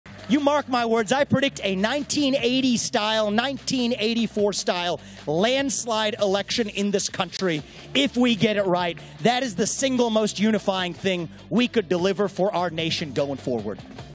CPAC 2023: Excellence over victimhood
Excellence over victimhood. Merit over grievance. Self-governance over aristocracy. That’s what it means to be American. If the GOP owns that, we win 2024 in a landslide. Vivek2024.com